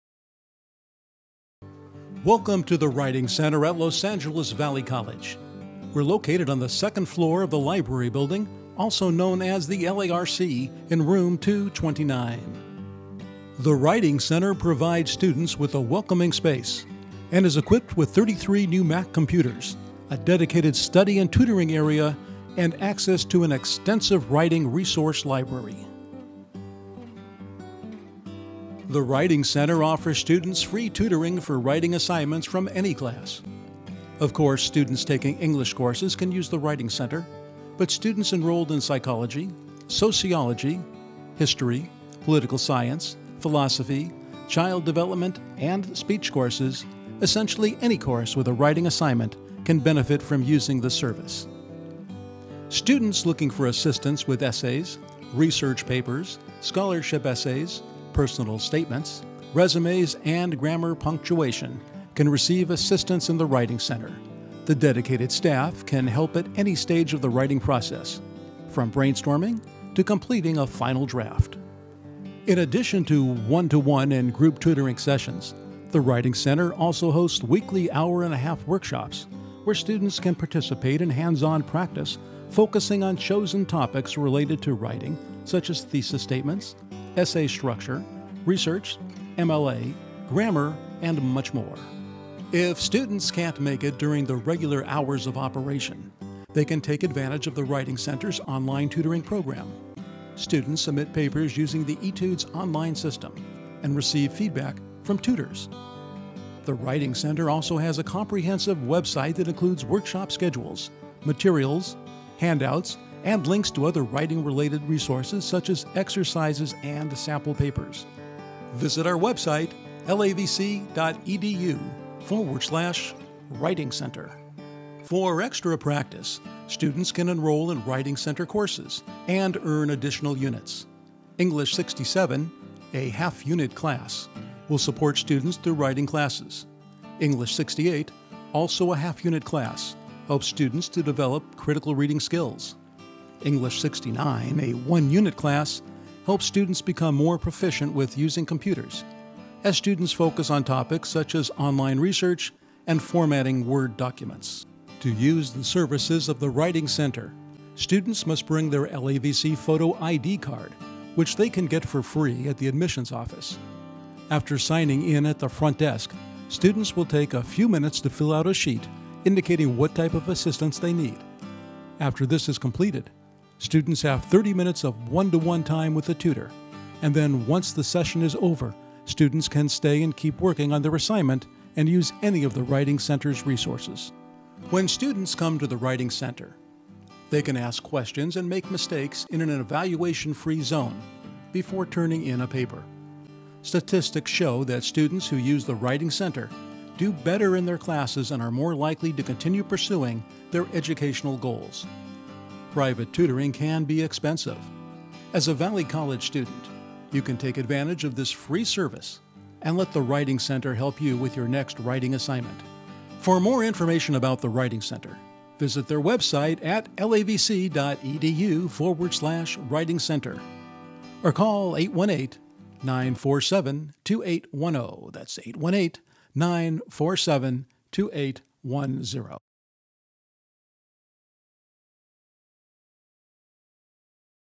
Writing Center at Los Angeles Valley College
This video provides an overview of the services offered at Los Angeles Valley College's Writing Center.